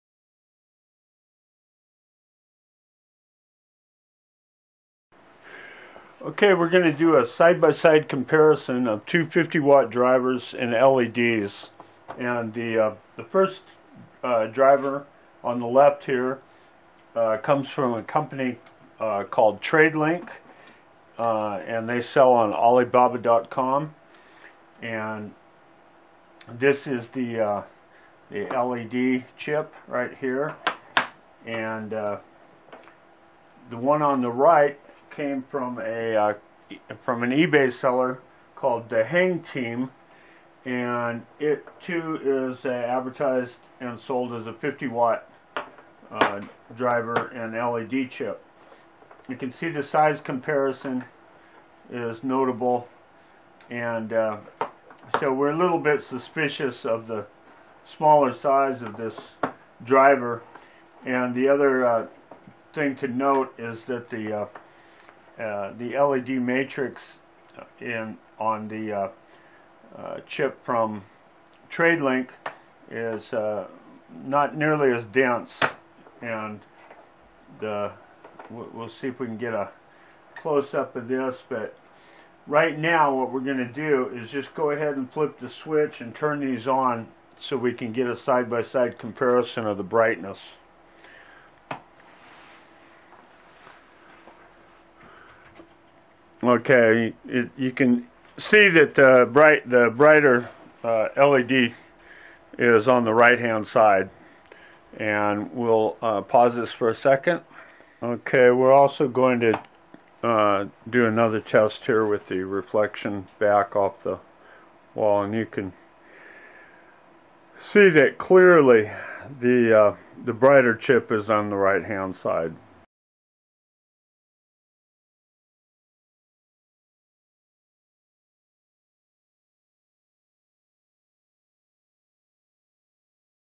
50 Watt LED Chip and Driver Comparison Number 1
HardluckCharlie shows a 50 Watt LED Chip and Driver Comparison.